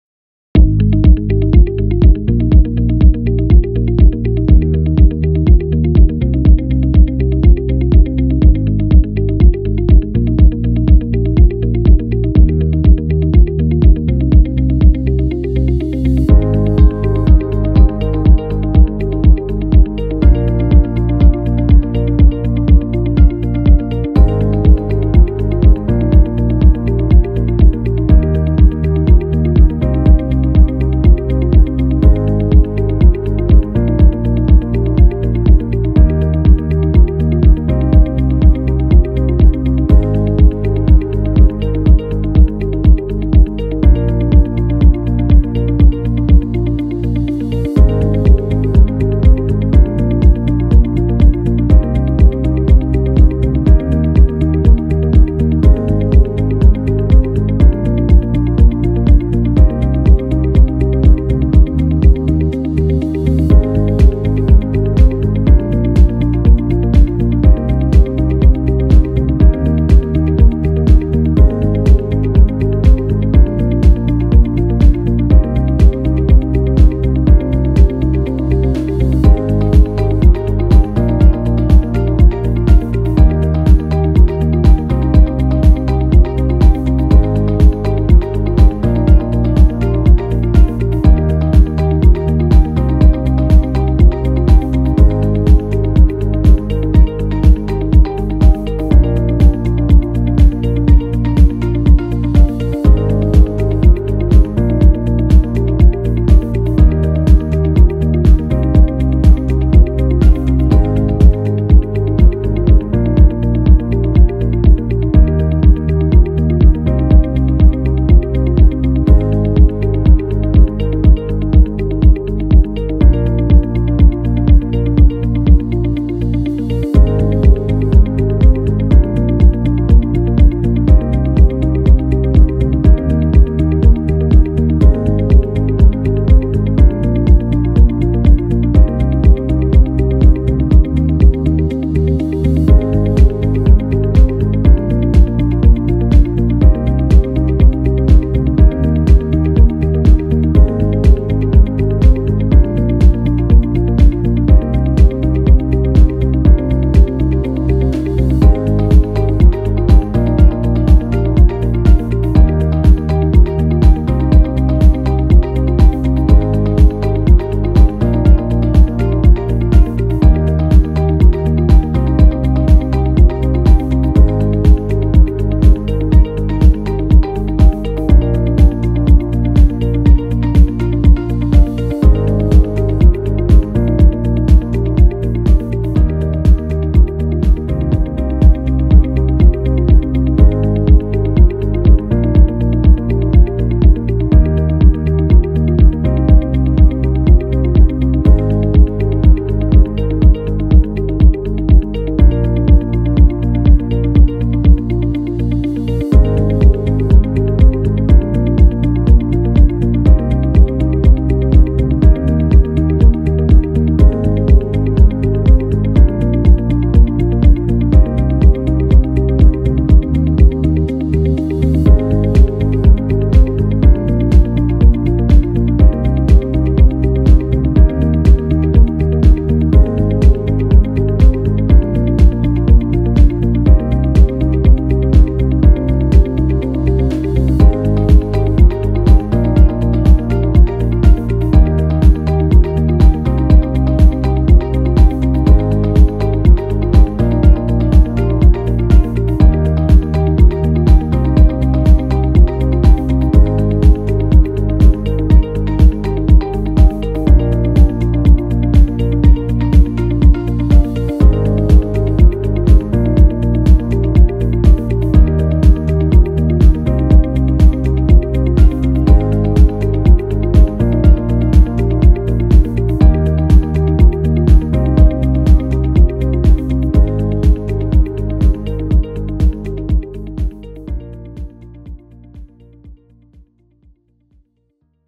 Real-life Motions for Public Transportation | Vehicle Interactions for iClone & ActorCore
Studio Mocap Series - Motions for Vehicle Interactions consists of several types of realistic movements based on common driving scenarios like getting in or getting off the car, idling, controlling the steering wheel, and more.

This pack includes multiple types of basic interactions between characters and different kinds of vehicles like SUVs, motorbikes, and pickups.